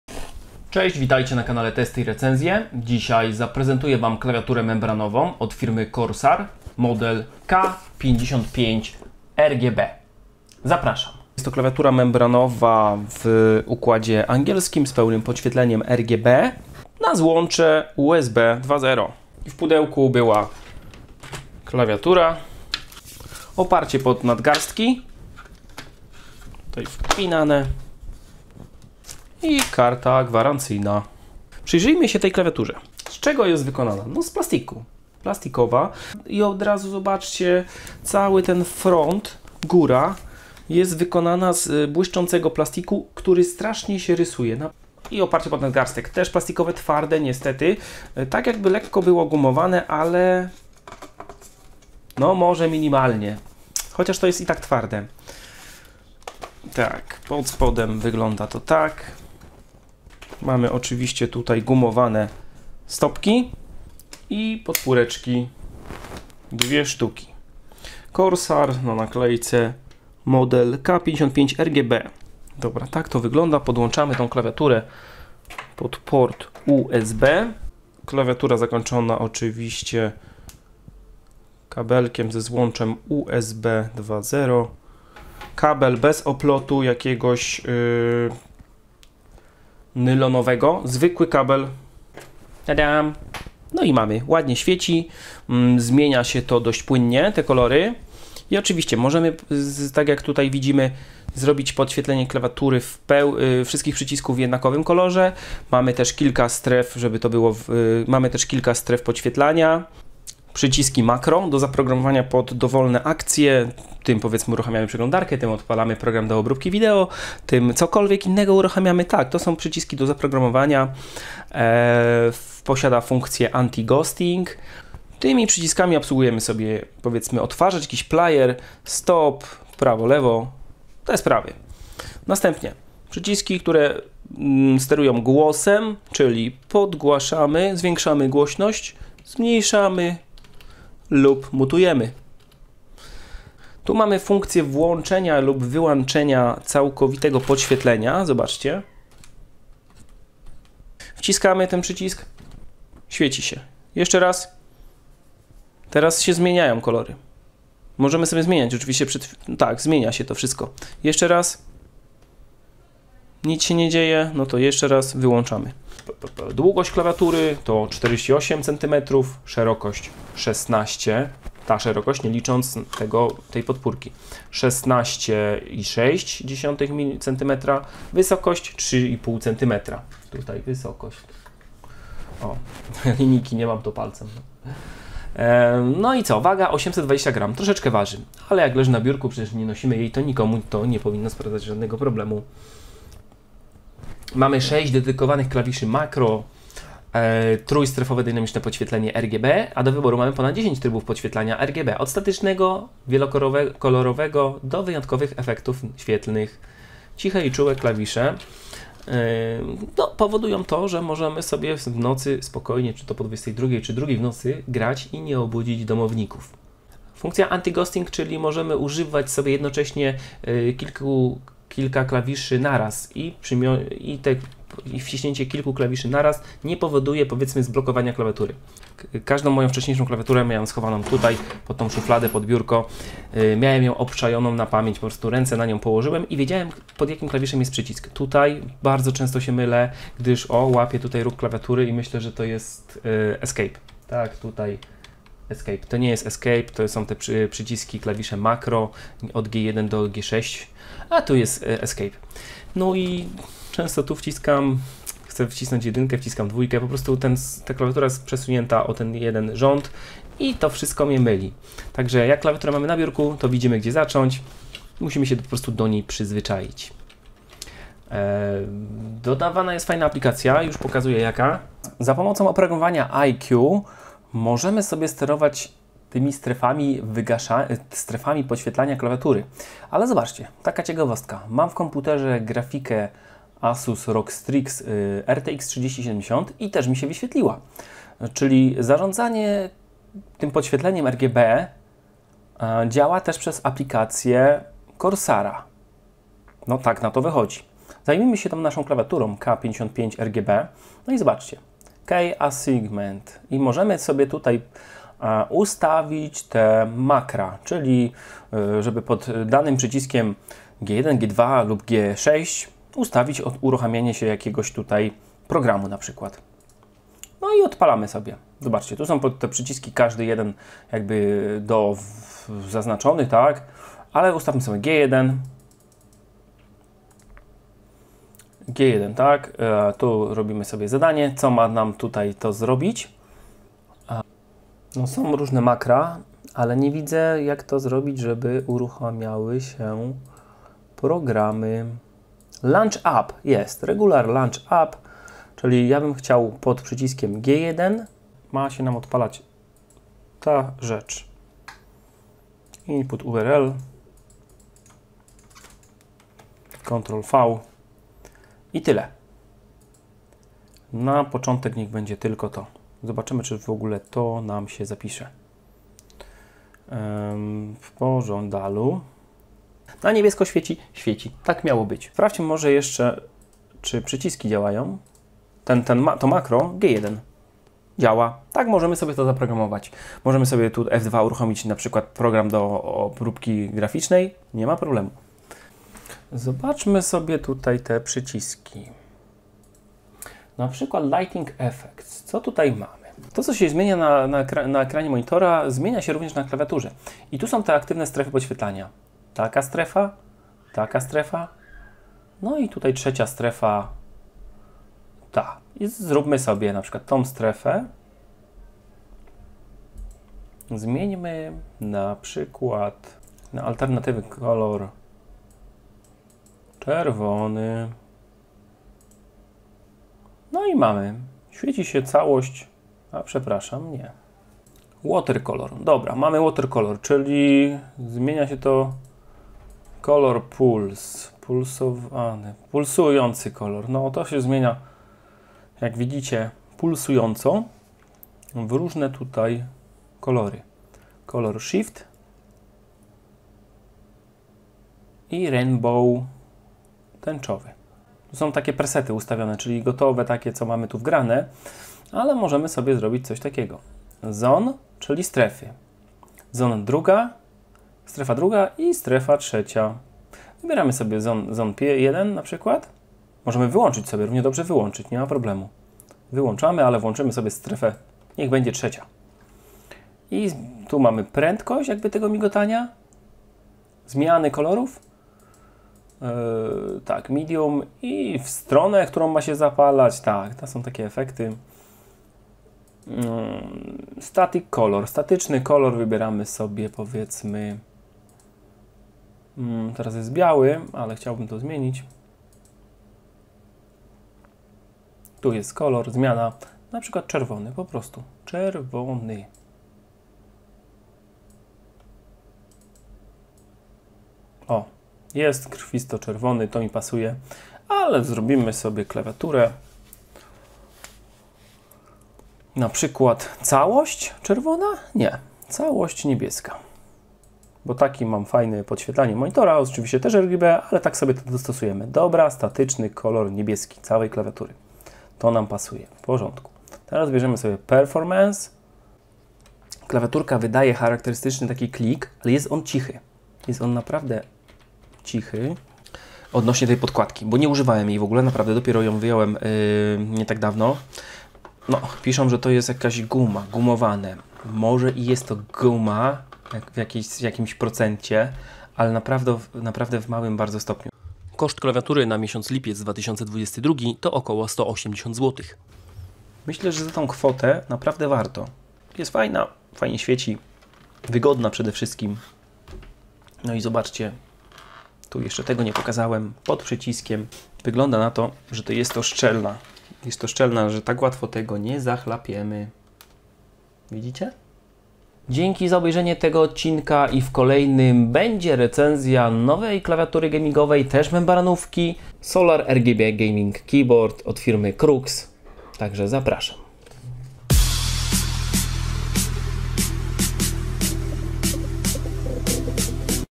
Recenzja klawiatury membranowej CORSAIR K55 z poświetlaniem RGB
Test klawiatury gamingowej membranowej z podświetleniem RGB marki Corsair.
🔥 Zostaw LAJKA jeśli JESTEŚ ze mną!
🔥 Zostaw SUBA jeśli CHCESZ BYĆ ze mną!